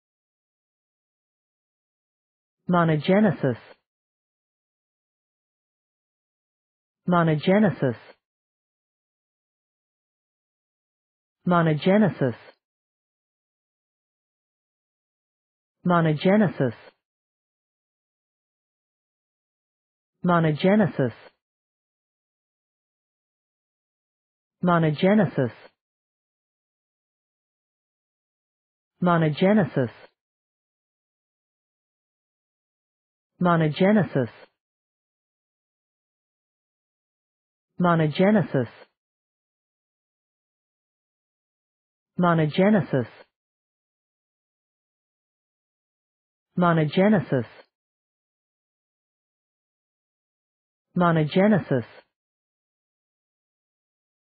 Pronunciation of Monogenesis | Definition of Monogenesis
Monogenesis pronunciation | How to pronounce Monogenesis in English?
/,mɑːnə`dʒenɪsɪs/
Meaning of Monogenesis | What is Monogenesis?
(noun) asexual reproduction by the production and release of spores
What is Monogenesis | Monogenesis meaning and definition?